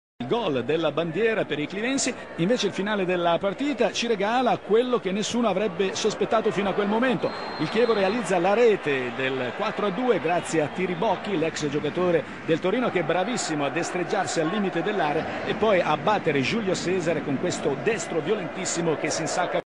Inter-Chievo gol di Tiribocchi
Gol di Simone Tiribocchi, contro l'inter